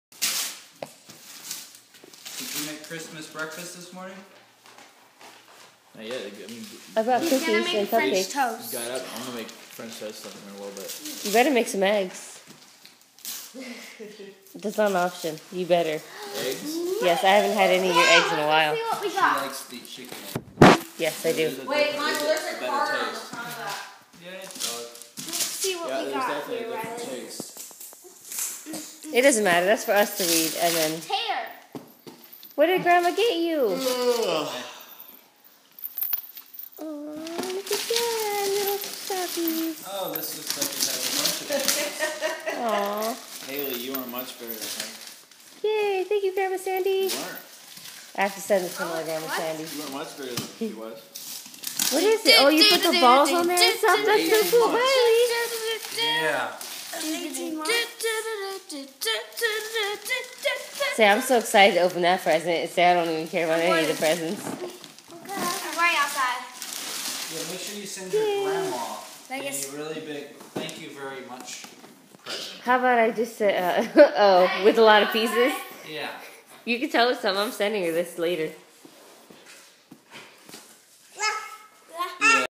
Just for Sandee
Rylee opening grandma Sandee's gift.